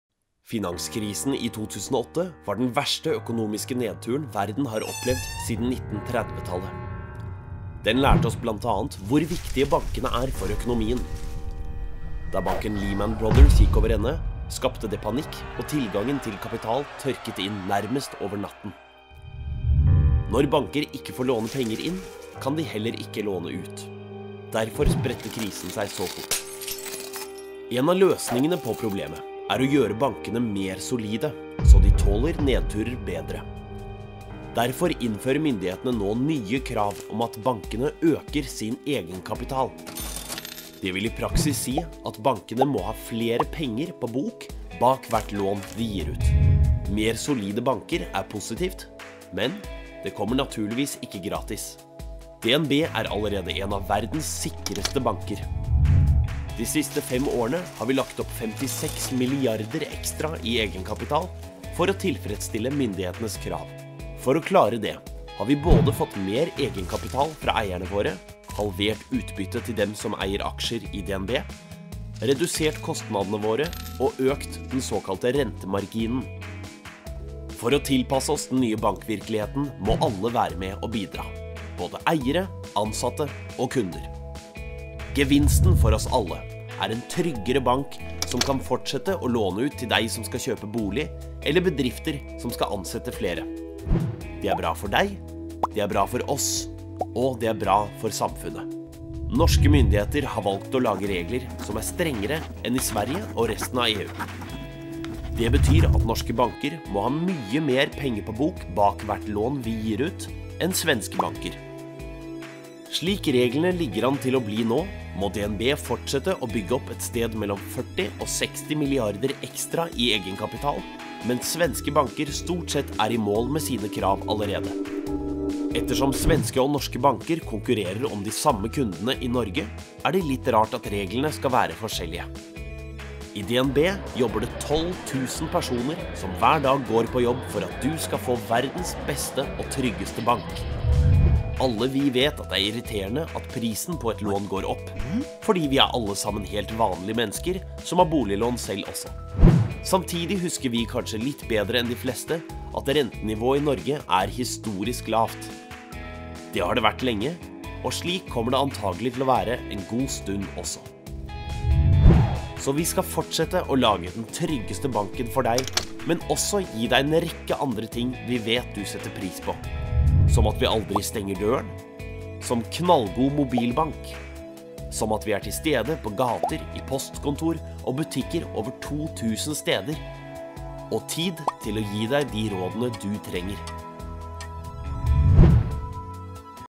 Ny bankvirkelighet
Hva innebærer ny bankhverdag, og hva betyr det for deg?